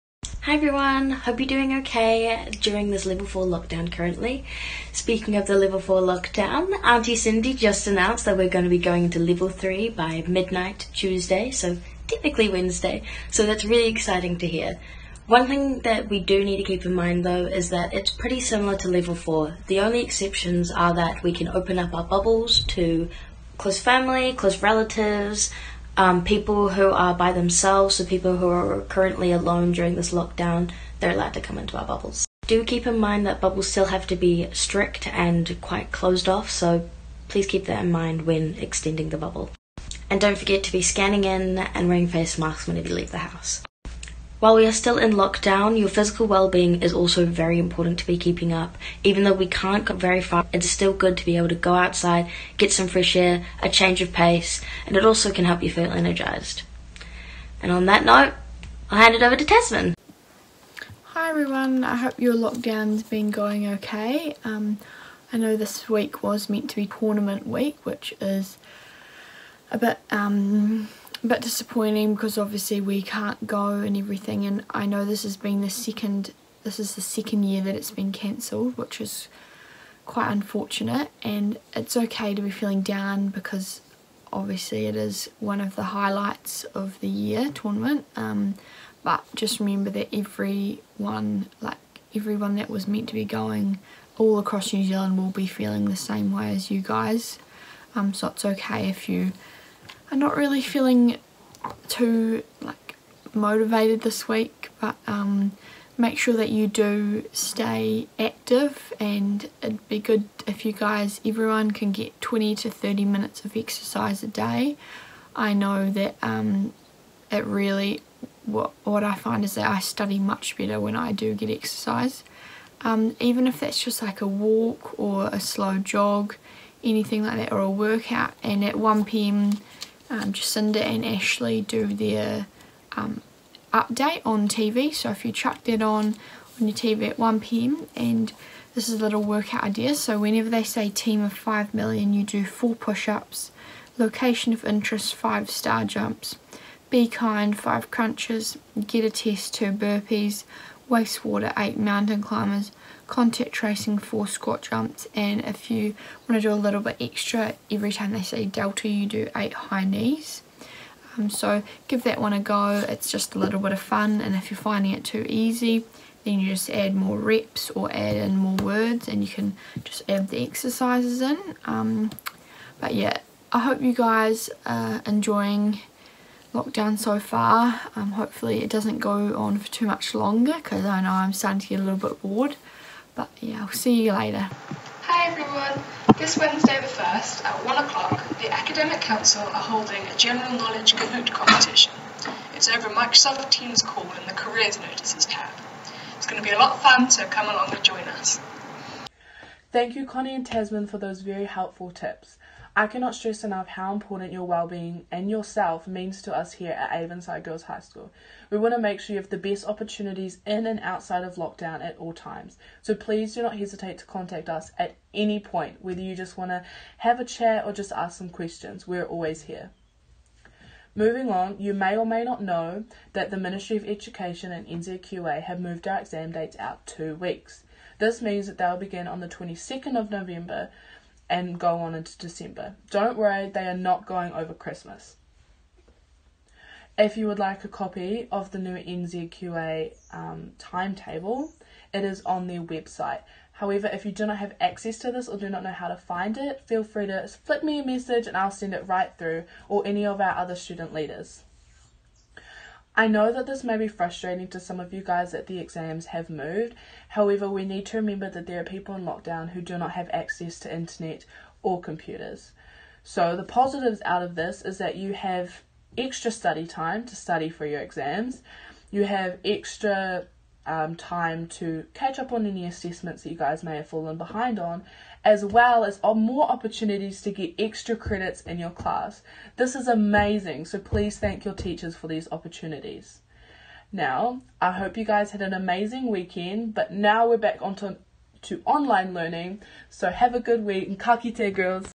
AGHS Aroha Edition 10: 31 August
The level 3 Avonside Aroha video is out.  Connie, Jade, Tasman and Aurora give you a bit of an update on what is happening and what you could be involved in.